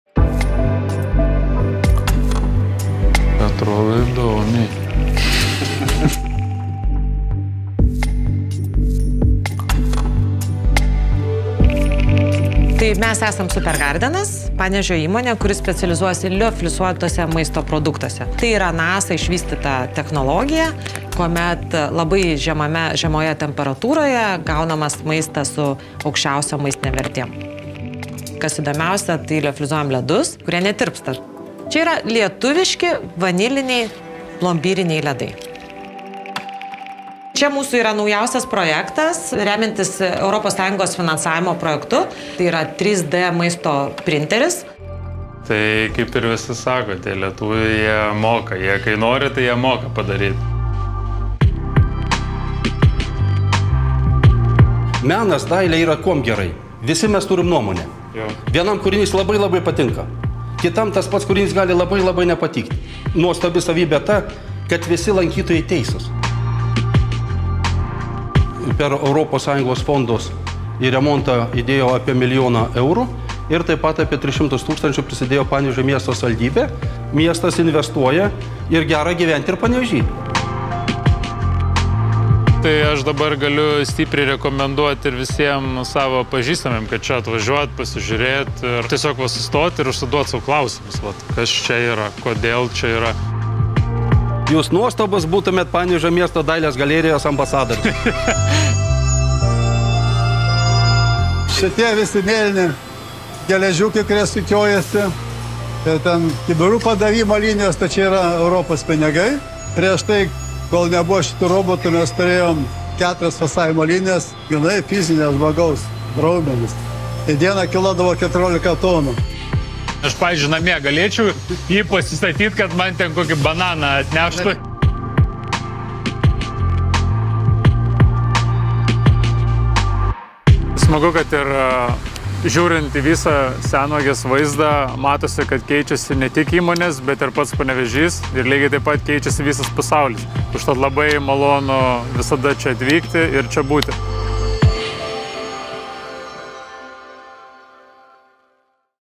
ES investicijų ambasadorius Panevėžyje - DANAS RAPŠYS
„Mano vaikystės Panevėžys tikrai atrodė daug liūdniau nei dabar. Mes patys jį kaimeliu vadindavom“, – pasakoja Molainių mikrorajone augęs plaukikas, Europos čempionas Danas Rapšys.